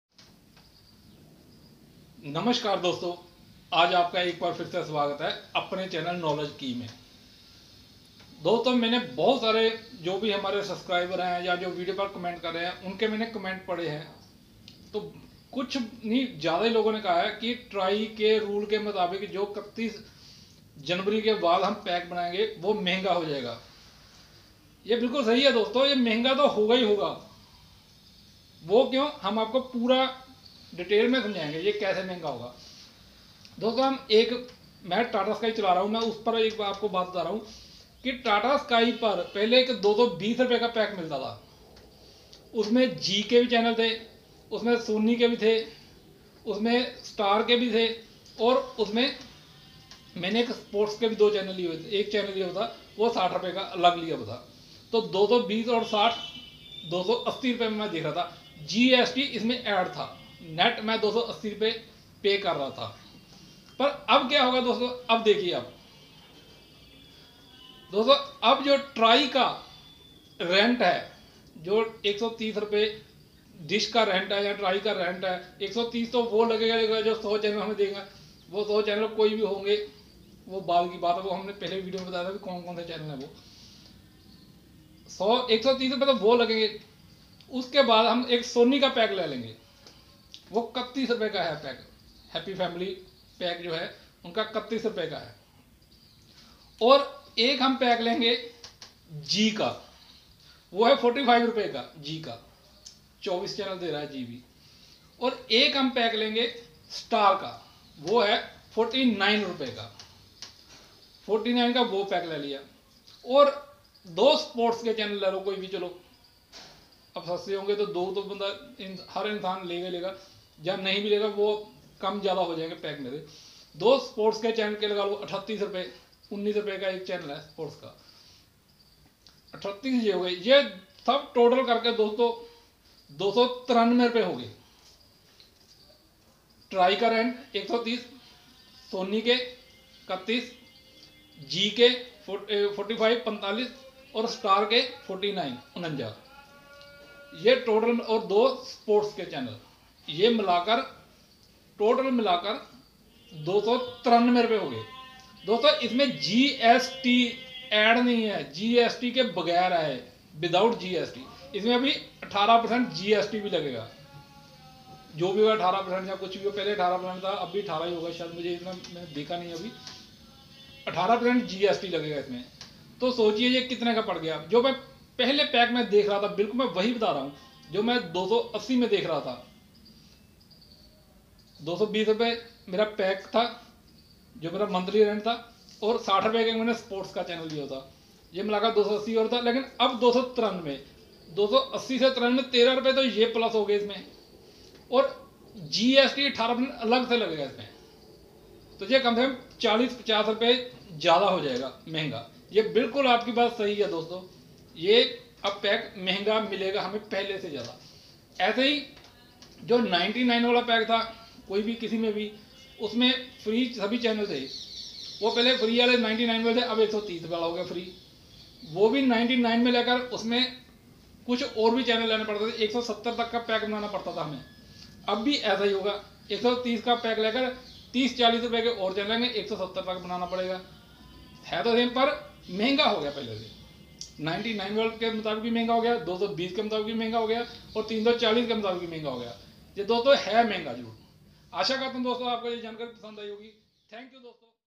new pack of all DTH TV & Cable TV will be costly after 31 january 2019
Estimate of NEW pack detail for all DTH TV & Cable TV

comparison of Tata sky dhamaka pack & TRAI Rule pack
TATA sky dhamaka pack price was 220rs with GST
& now estimate of trai rule pack
TRAI RENT.        -130
SONY PACK.      -  31
STAR PACK.       -  49
ZEE PACK.          -  45
2 SPORTS PACK-  38
                              293
Total 293 without GST